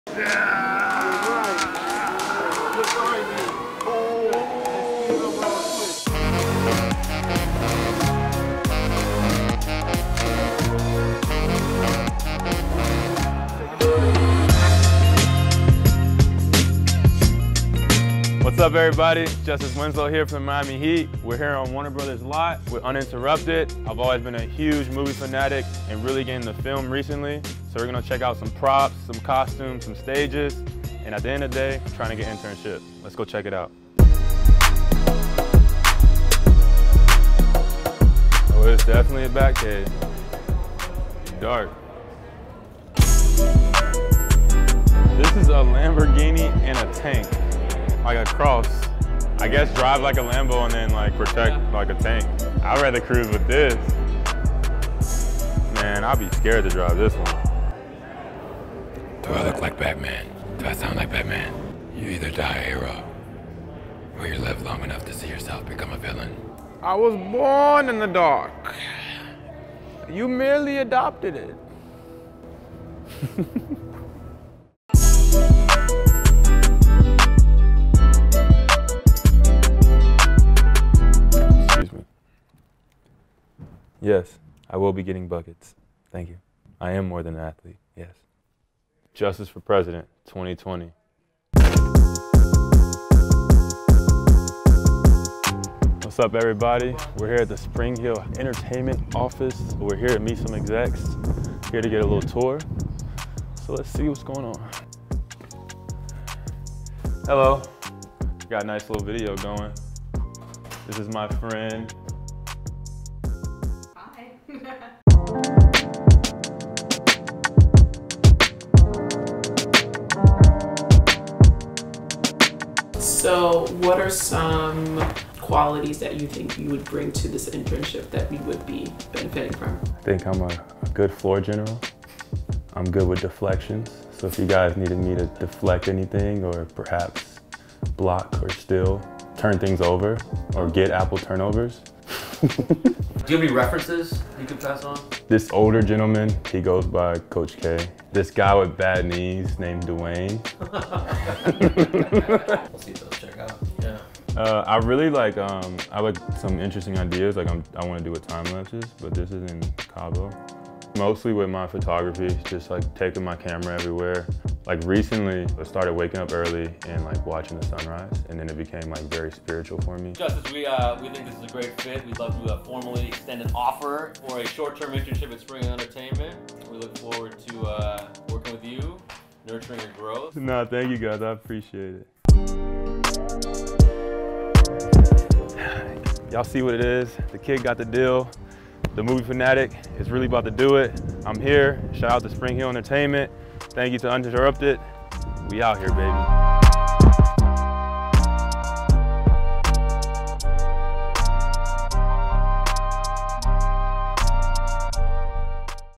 Justise Winslow geeks out at Warner Bros studio lot | Extracurriculars
NBA wing, movie buff, and…Batman? Miami Heat wing Justise Winslow spent a day on the Warner Bros studio lot to go behind the scenes and try to break into the entertainment industry. 🎬🎞🎥